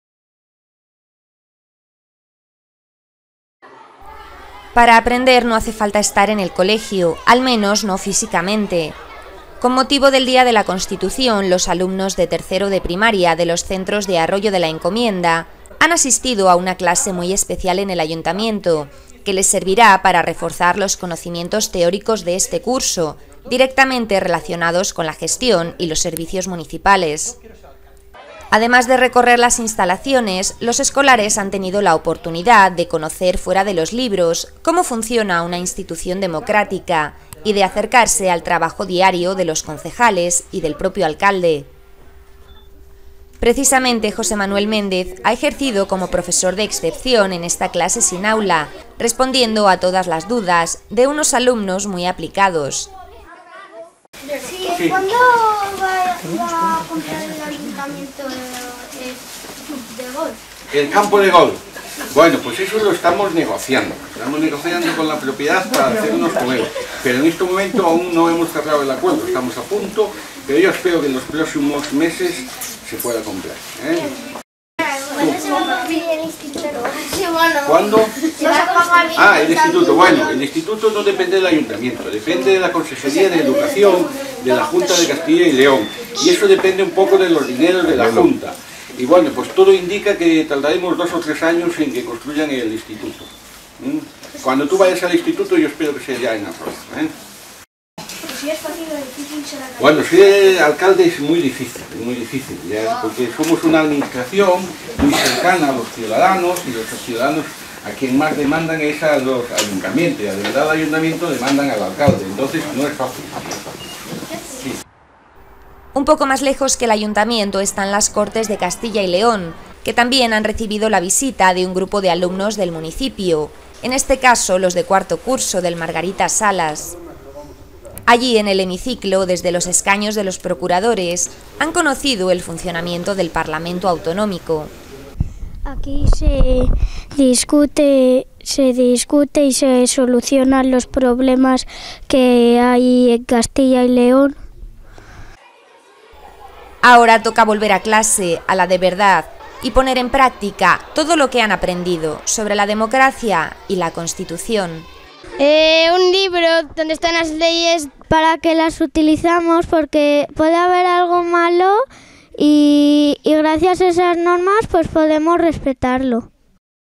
Día de la Constitución
Noticia del periódico digital de Arroyo de la Encomienda Arroyo al Día Los escolares de Arroyo de la Encomienda visitan el Ayuntamiento del municipio y las Cortes de Castilla y León con motivo del Día de la Constitución.